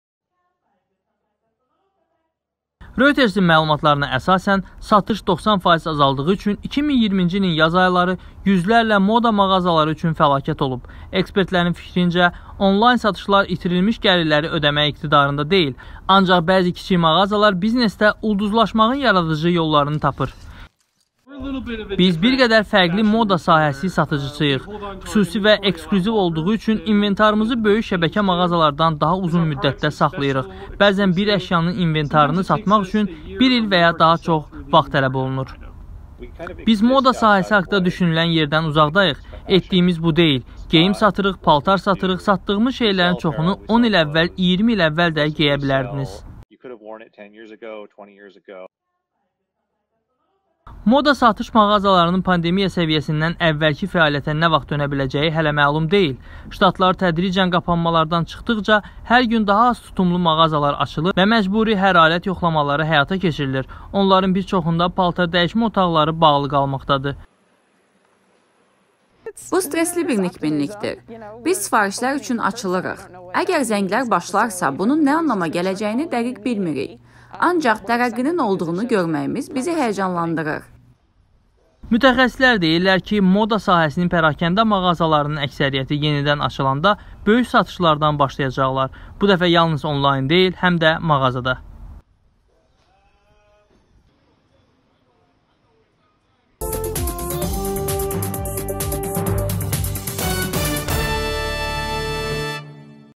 Dəb sahəsinin mağazaları iqtisadi böhrandan çıxmağa çətinlik çəkir
Ştatların bir-birinin ardınca qapanmadan çıxmasına baxmayaraq, moda sənayesi mağazaları ciddi problemlə üzləşir - aylardır rəflərdə qalan malların satıla bilməməsi ilə.